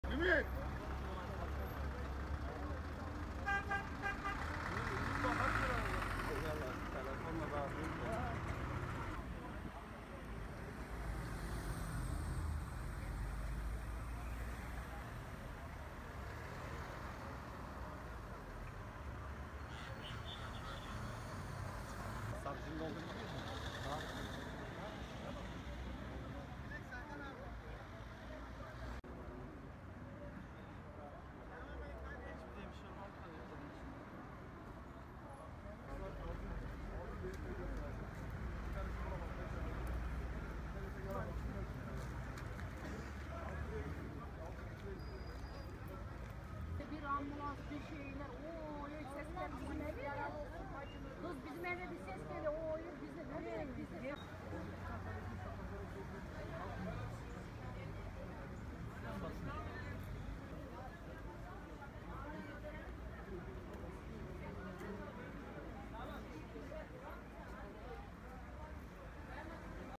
Adana'da dolmuş durakta bekleyenlere çarptı: 1 ölü, 7 yaralı
Adana'da dolmuşun otobüs durağında bekleyenlere çarpması sonucu 1 kişi hayatını kaybetti, 7 kişi yaralandı.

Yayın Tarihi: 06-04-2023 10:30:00